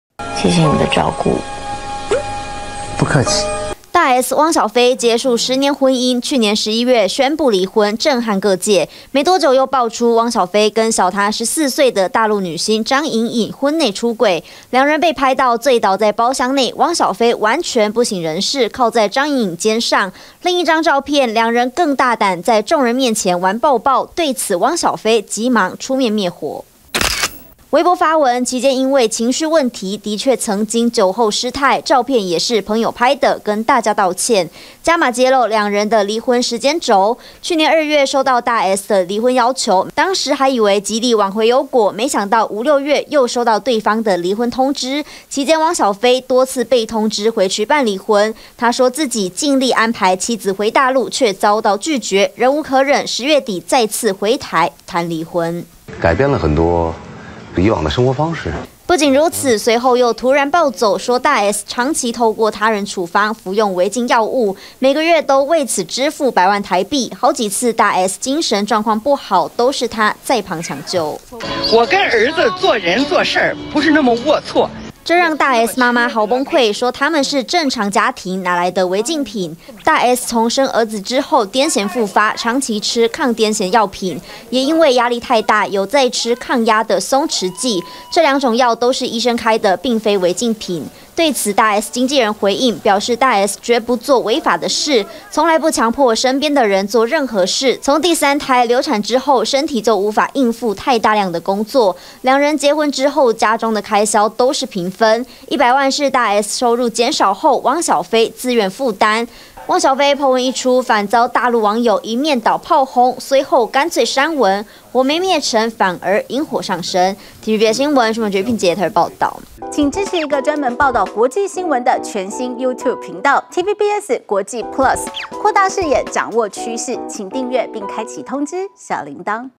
汪小菲爆大S服禁藥 S媽澄清是癲癇藥｜TVBS新聞
汪小菲今天(5/30)PO文，對於婚內曾和女星張穎穎抱抱，他滅火自稱酒後失態，但否認出軌，還加碼爆料婚變時間軸，指控大S長期服用違禁藥物，他每個月為此支付百萬台幣，對此大S經紀人澄清，表示大S絕不做違法的事，大S媽媽則反擊，那些是抗癲癇藥物，請汪小菲留些口德。